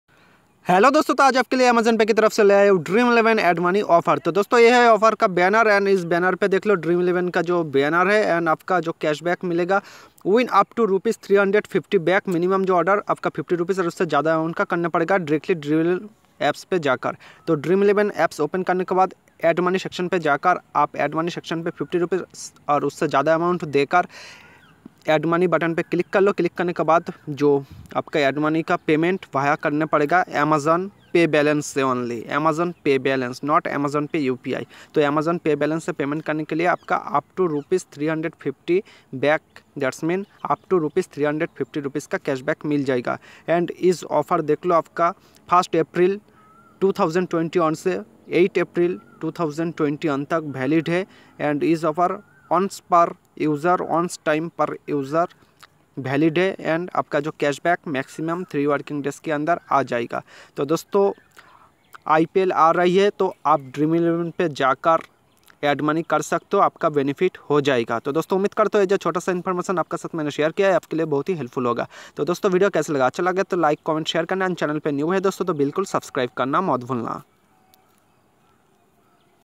Amazon Pay Dream11 Add Money Offer of April,2021! Win UpTo ₹350/- Cash Back Offer!
👉If Our Telegram Channel's Link Is Not Working,Pls Search "Study Challenger" In Your Telegram Search Box & Then Join It!